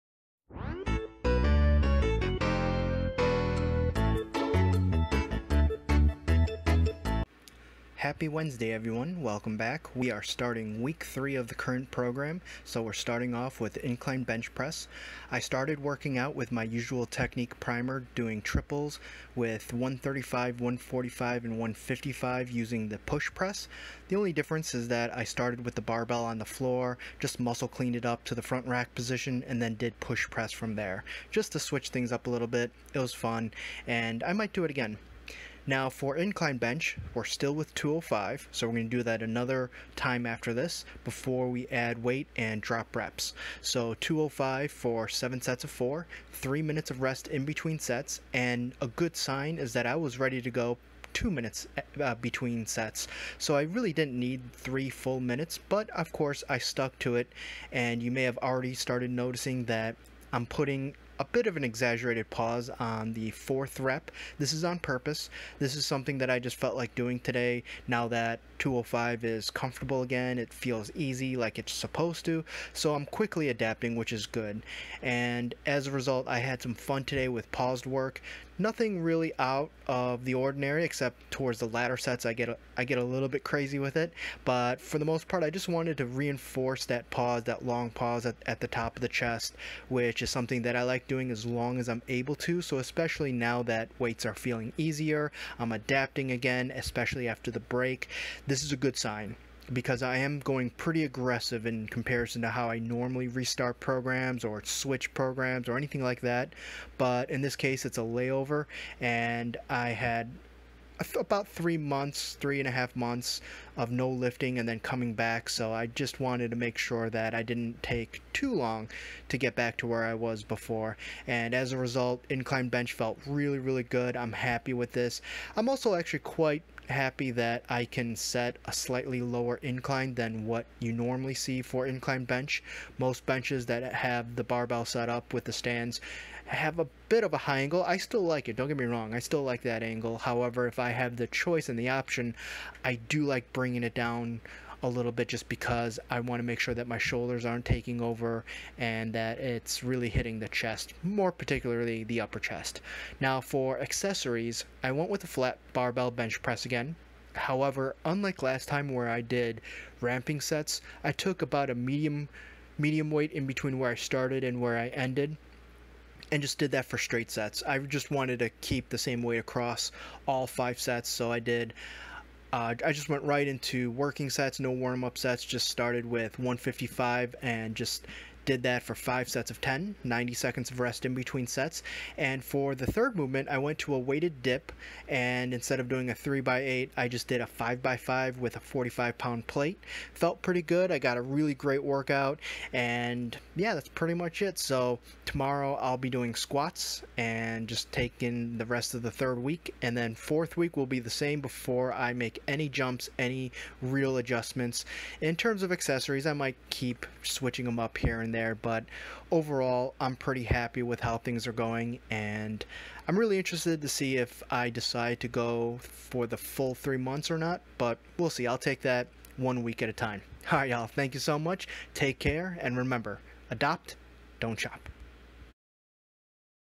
9/15/21 Incline Bench Press
3rd week is off to a great start. Weighted Dip was the last exercise.

Muscle Clean + Push Press
135lb x 3
145lb x 3
155lb x 3

155lb x 5
185lb x 4
205lb x 4
205lb x 4
205lb x 4
205lb x 4
205lb x 4
205lb x 4
205lb x 4

Flat Barbell Bench Press
155lb x 10
155lb x 10
155lb x 10
155lb x 10
155lb x 10

Weighted Dip
45lb x 5
45lb x 5
45lb x 5
45lb x 5
45lb x5